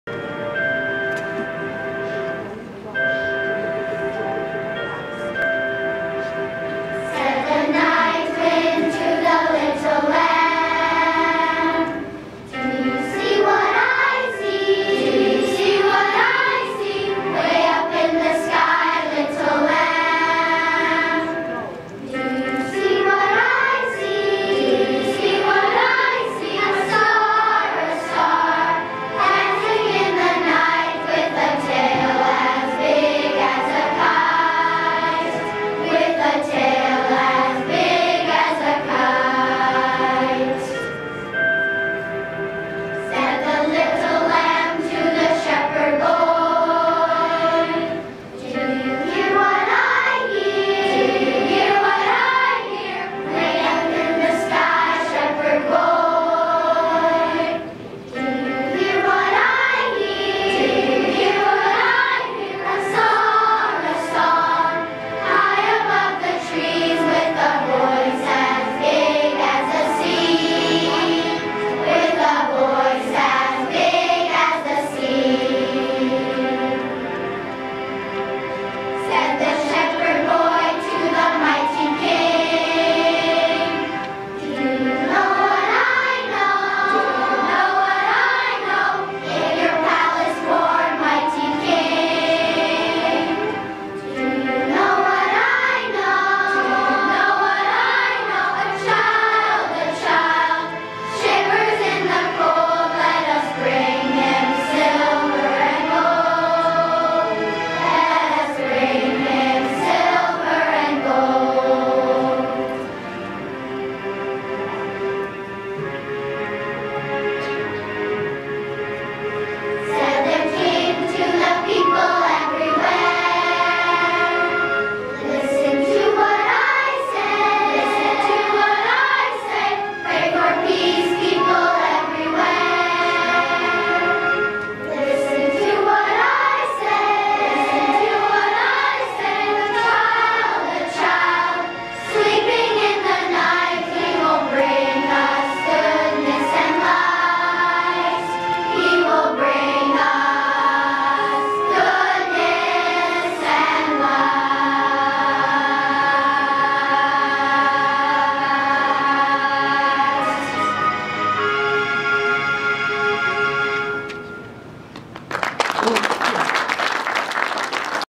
Promontory Elementary School Choir Song 1
The Promontory Elementary School Choir sang Christmas carols at the December 19, 2017 Council meeting.